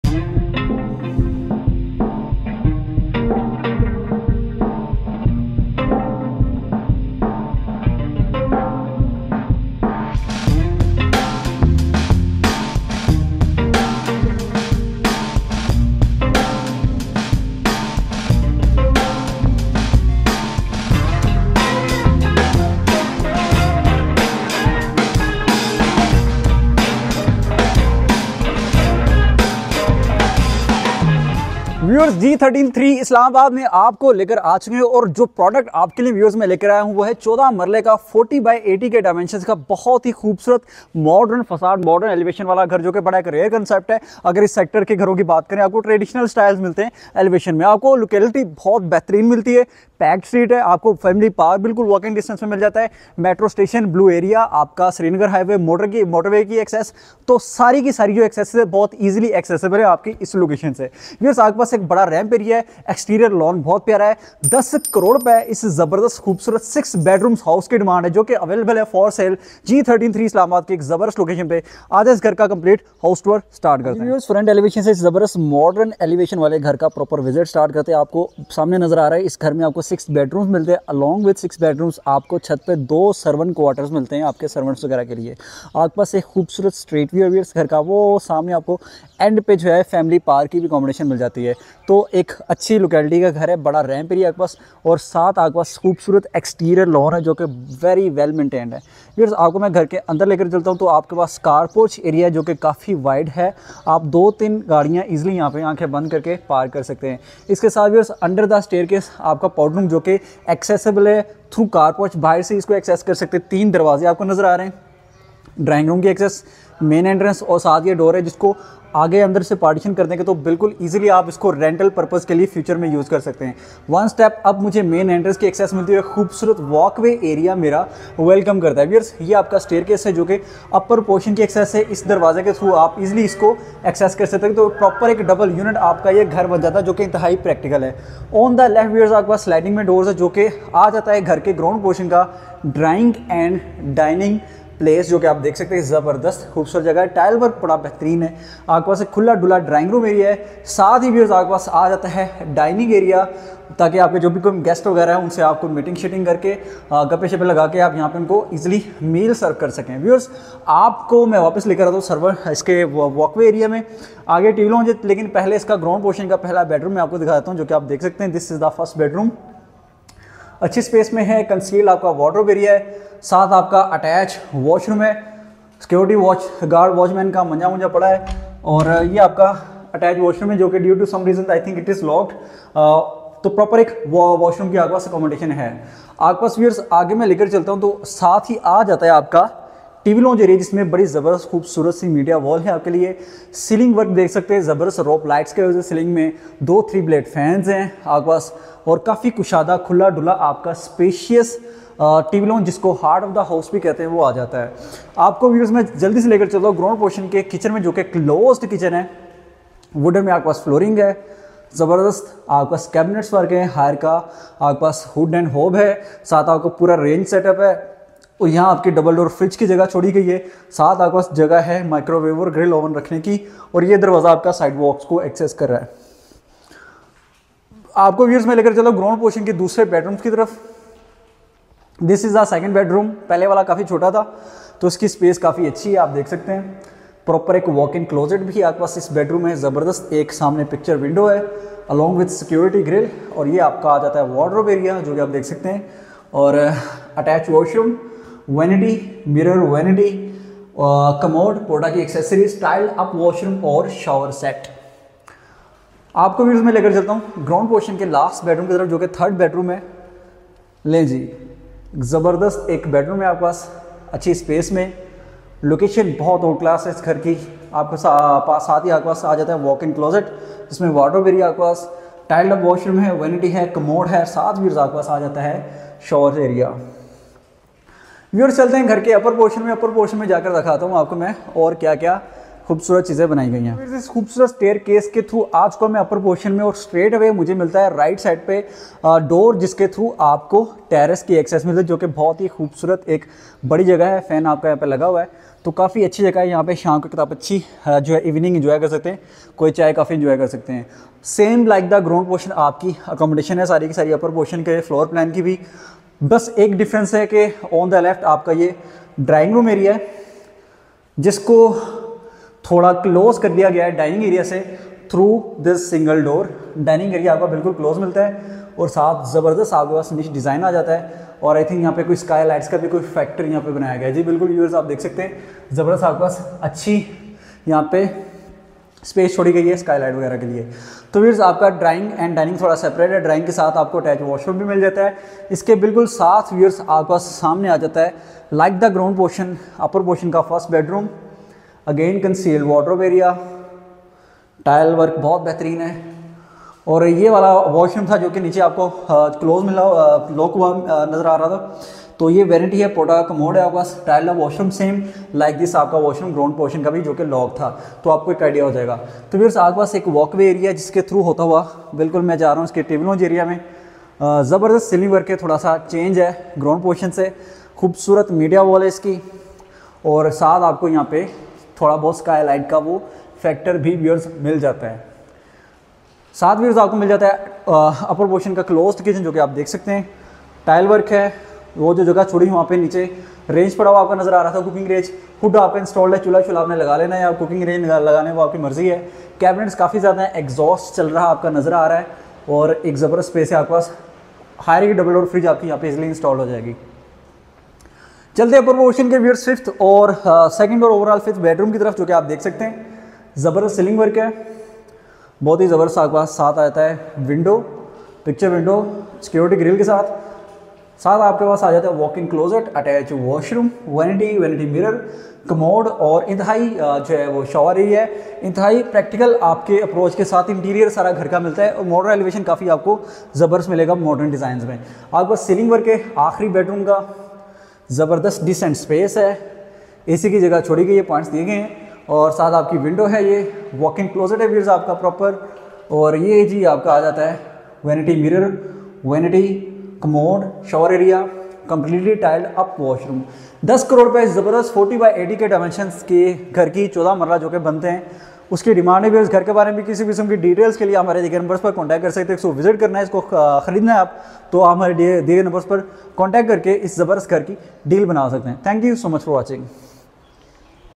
14 Marla HIGHLY-RECOMMENDED Brilliant House For Sale G-13 Islamabad
14 Marla HIGHLY-RECOMMENDED Brilliant House For Sale G-13 Islamabad

Key Features:

• Beautiful Prime Location with family park on walking distance
• Modern Front Facade
• Wide car porch
• Powder room
• Drawing room
• Dinning room
• 6 bedrooms with attached bathrooms
• TV Lounge
• Kitchen
• Laundry area
• Servant quarter with bathroom